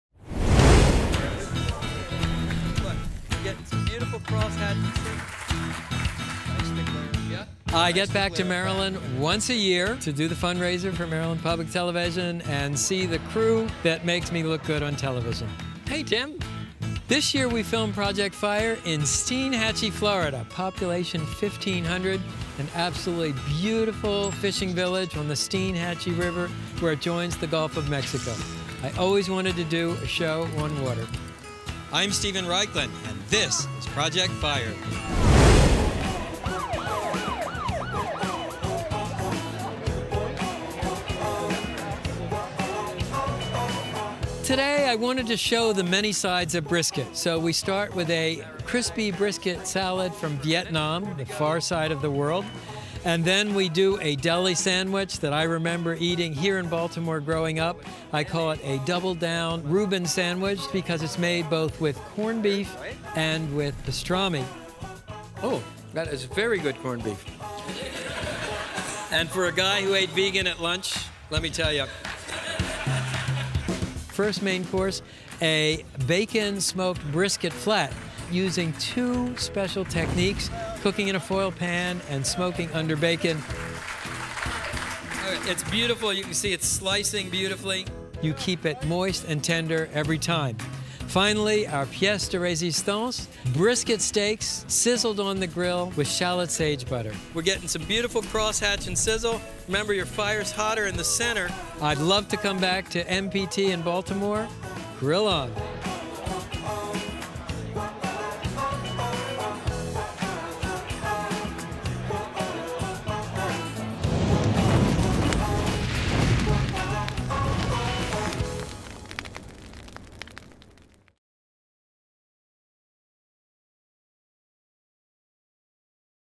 Steven Raichlen Master Class Behind-the-Scenes interview
MPT Members learned how to take their grilling to the next level with James Beard winner and BBQ hall of fame winner Steven Raichlen. Raichlen's Master Class was held at Maryland Public Television on June 8 2019 for a bustling crowd of foodies, chefs, and meat lovers. Learn more about how Raichlen perfects the art of brisket-making and catch behind-the-scenes secrets of season 2 of Project Fire. Interview conducted by Sherryn Daniel.